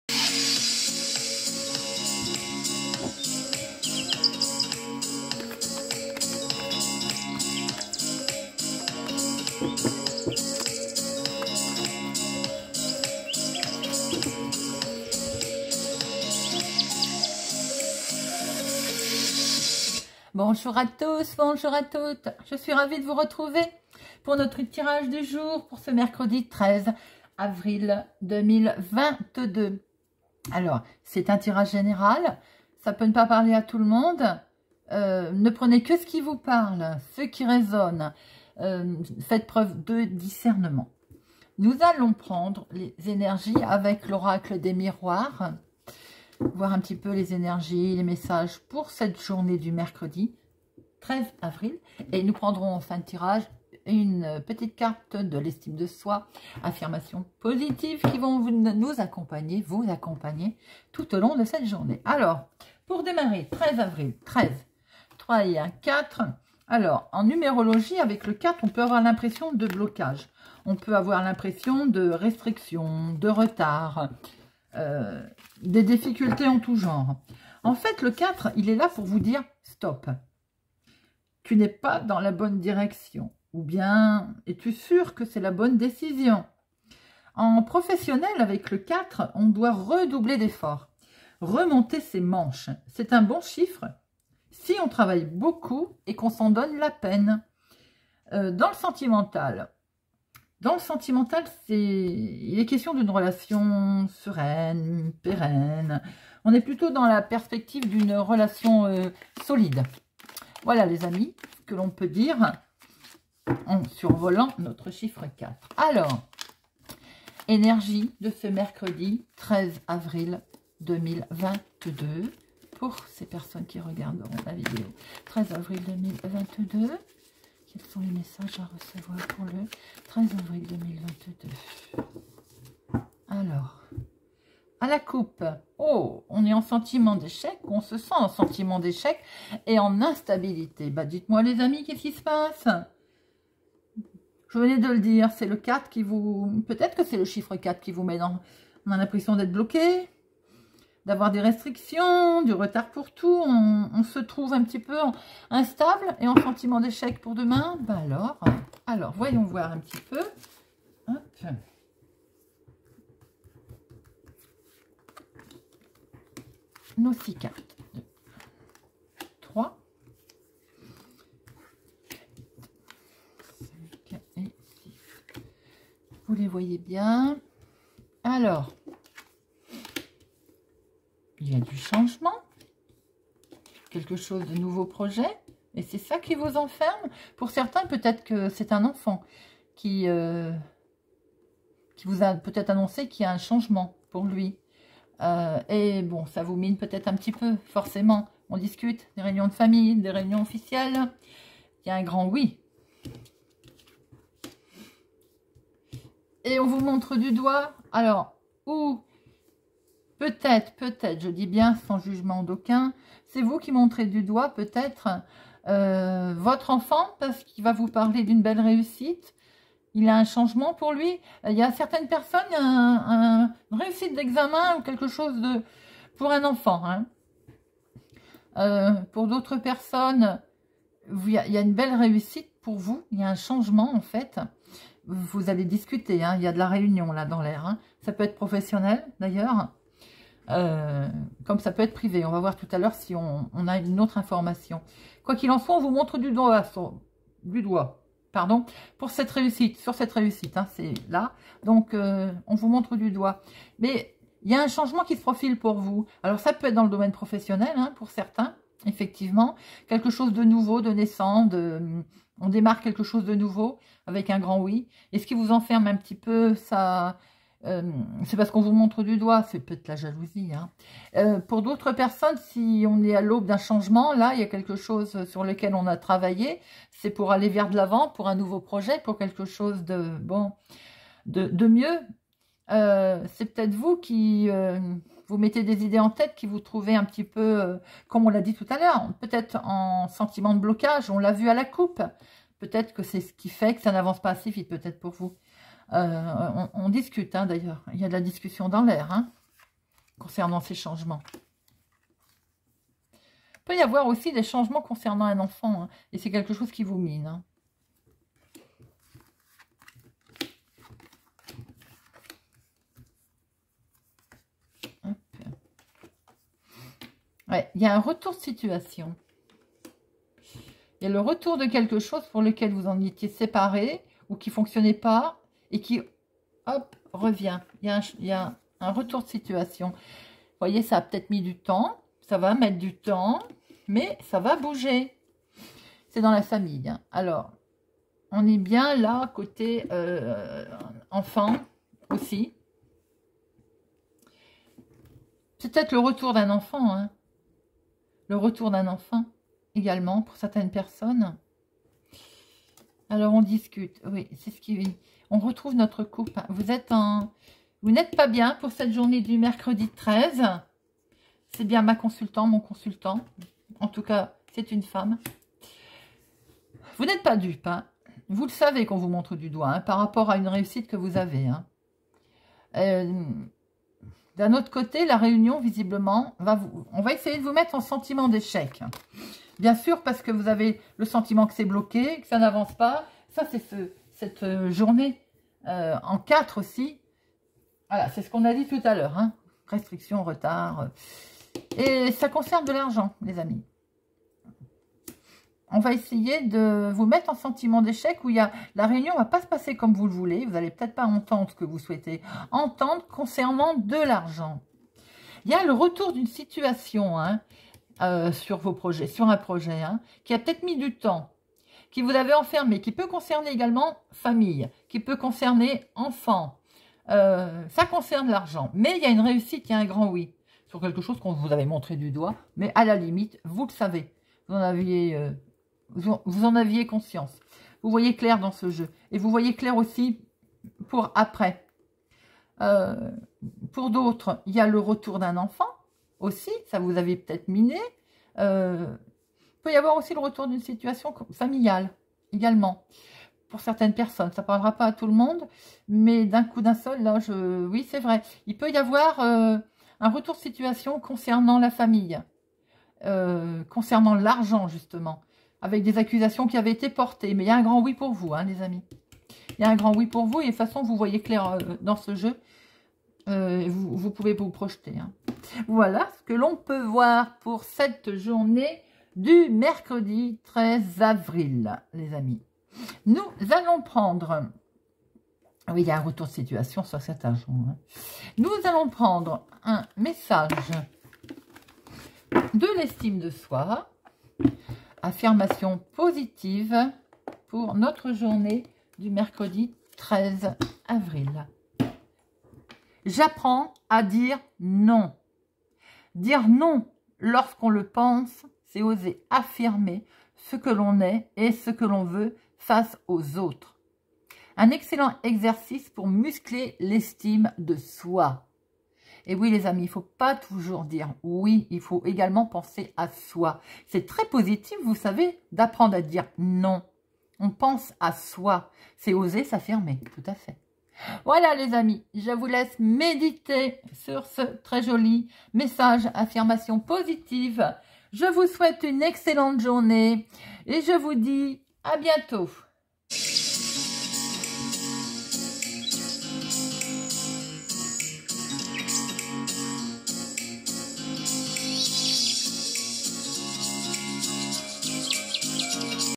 Tirage général du jour Mercredi 13 Avril 2022 ( changement, réussite, mais !!!!! )😗
Tirage général du jour Mercredi 13 Avril 2022 avec l oracle des miroirs et un message de l estime de soi.

C'est un tirage général ça peut ne pas parler à tout le monde ne prenez que ce qui résonne en vous 😘

Merci d être présent sur ma chaine ❤️
Merci pour vos commentaires et votre fidélité 🙏